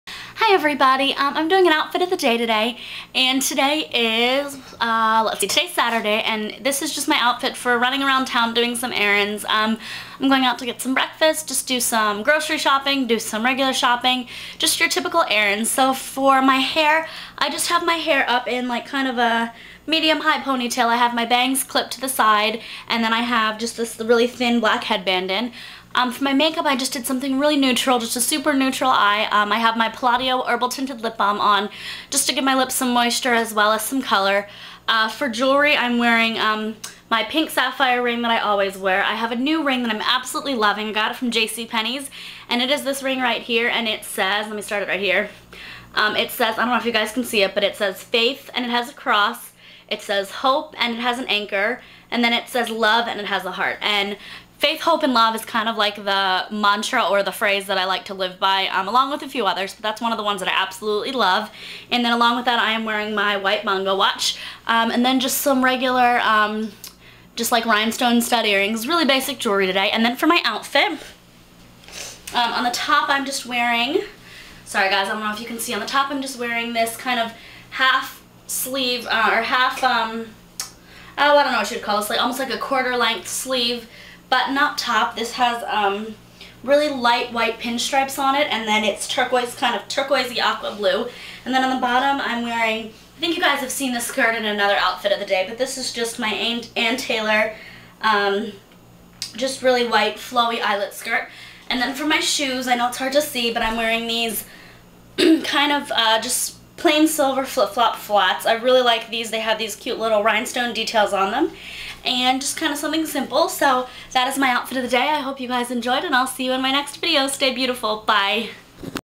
Casual Outfit of The Day- 10/9/10
This my outfit of the day, just a typical Saturday!
Watch- White Bongo watch
Top- New York and Company
Skirt- Ann Taylor
Shoes- Target